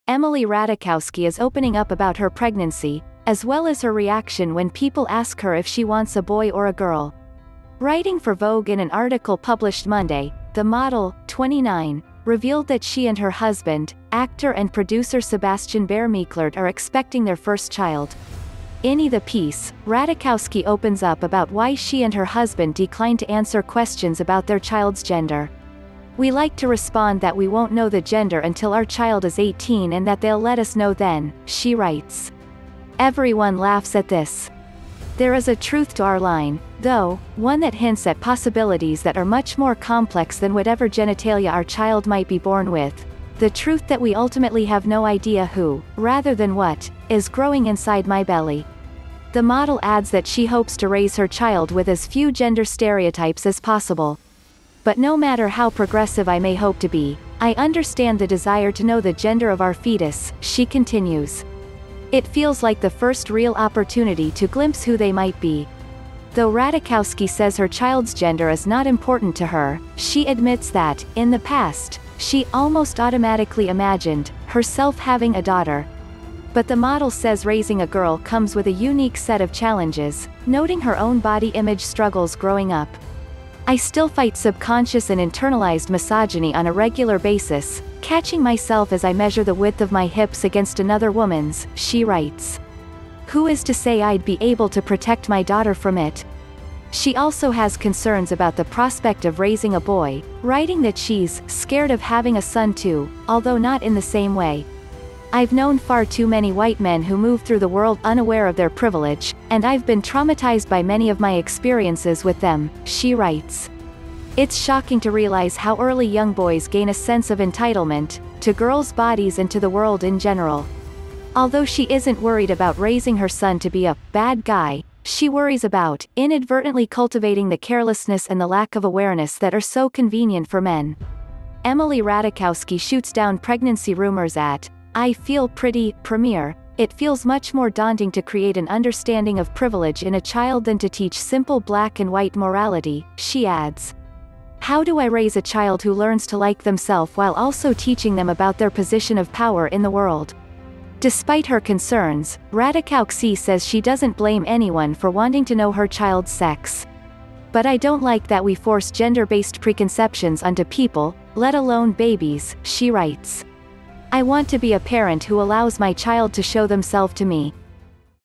Surprise! Emily Ratajkowski reveals pregnancy why she isn't announcing
Emily Ratajkowski is opening up about her pregnancy – as well as her reaction when people ask her if she wants a boy or a girl. Writing for Vogue in an article published Monday, the model, 29, revealed that she and her husband, actor and producer Sebastian Bear-McClard are expecting their first child. Ini the piece, Ratajkowski opens up about why she and her husband decline to answer questions about their child's gender.''We like to respond that we won’t know the gender until our child is 18 and that they’ll let us know then, '' she writes. ''Everyone laughs at this. There is a truth to our line, though, one that hints at possibilities that are much more complex than whatever genitalia our child might be born with: the truth that we ultimately have no idea who – rather than what – is growing inside my belly. ''The model adds that she hopes to raise her child with as few gender stereotypes as possible.''But no matter how progressive I may hope to be, I understand the desire to know the gender of our fetus, '' she continues. ''It feels like the first real opportunity to glimpse who they might be. ''Though Ratajkowski says her child's gender is not important to her, she admits that, in the past, she ''almost automatically imagined'' herself having a daughter. But the model says raising a girl comes with a unique set of challenges, noting her own body image struggles growing up.''I still fight subconscious and internalized misogyny on a regular basis, catching myself as I measure the width of my hips against another woman’s, '' she writes. ''Who is to say I’d be able to protect my daughter from it? ''She also has concerns about the prospect of raising a boy, writing that she's ''scared of having a son too, although not in the same way. ''''I’ve known far too many white men who move through the world unaware of their privilege, and I’ve been traumatized by many of my experiences with them, '' she writes. ''It’s shocking to realize how early young boys gain a sense of entitlement – to girls’ bodies and to the world in general. ''Although she isn't worried about raising her son to be a ''bad guy, '' she worries about ''inadvertently cultivating the carelessness and the lack of awareness that are so convenient for men. ''Emily Ratajkowski shoots down pregnancy rumors at 'I Feel Pretty' premiere''It feels much more daunting to create an understanding of privilege in a child than to teach simple black-and-white morality, '' she adds. ''How do I raise a child who learns to like themself while also teaching them about their position of power in the world? ''Despite her concerns, Ratajkowksi says she doesn't blame anyone for wanting to know her child's sex.''But I don’t like that we force gender-based preconceptions onto people, let alone babies, '' she writes. ''I want to be a parent who allows my child to show themself to me.